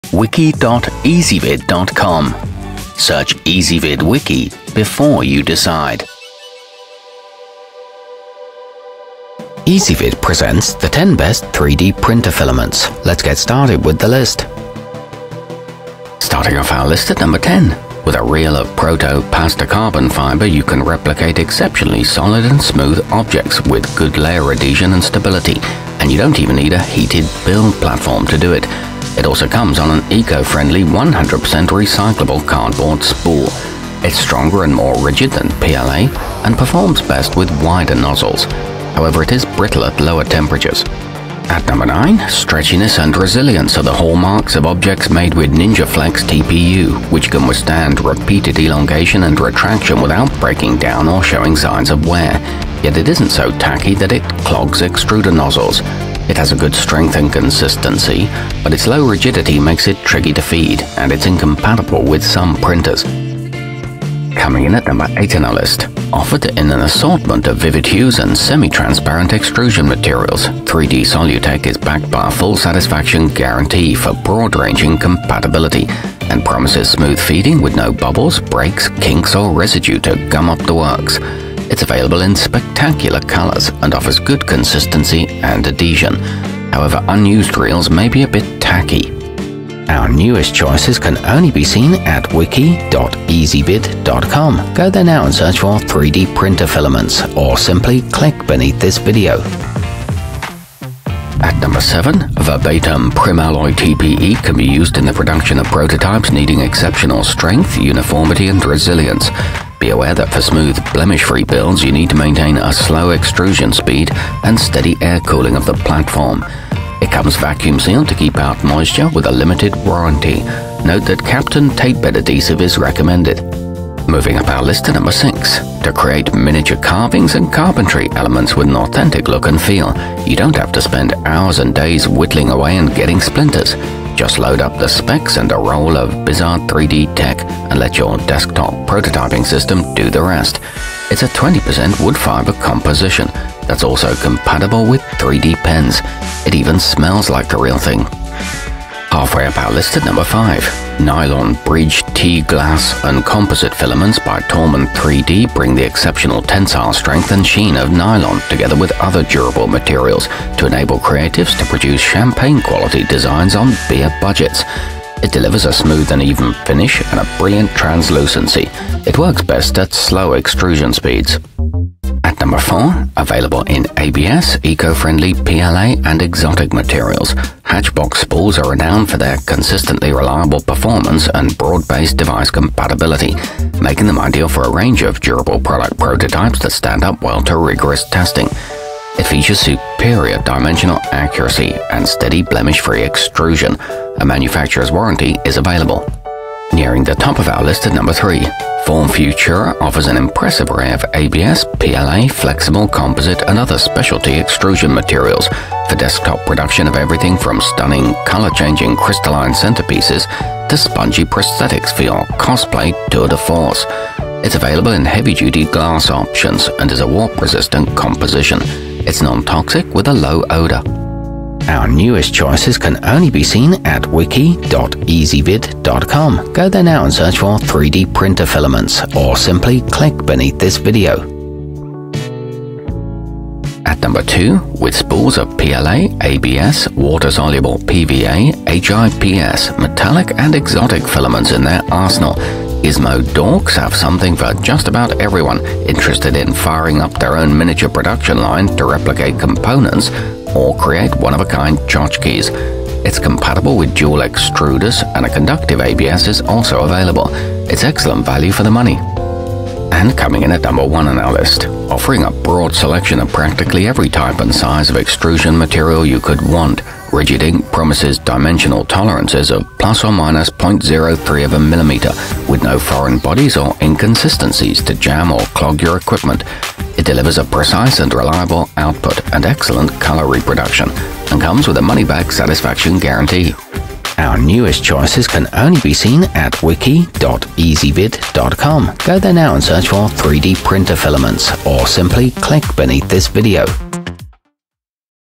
10 Best 3D Printer Filaments 2018
Our complete review, including our selection for the year's best 3d printer filament, is exclusively available on Ezvid Wiki.

3d printer filaments included in this wiki include the bizard 3d tech, form futura, gizmo dorks, 3d solutech, ninjaflex tpu, hatchbox spools, proto-pasta carbon fiber, verbatim primalloy tpe, rigid ink, and taulman 3d.